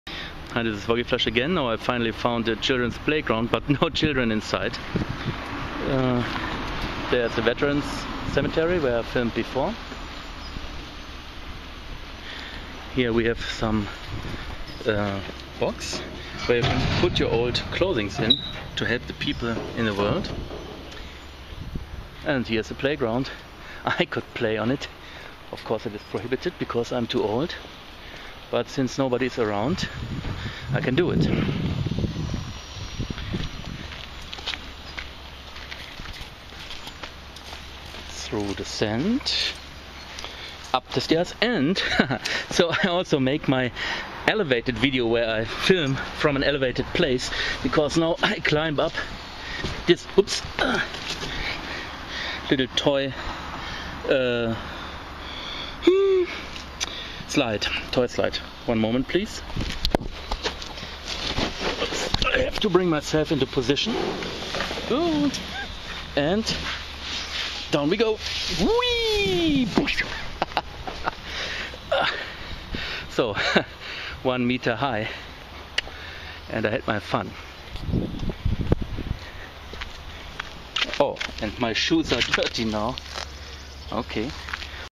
Me riding a slide on a children's playground
Of course it is VERBOTEN (prohibited) but it was great fun!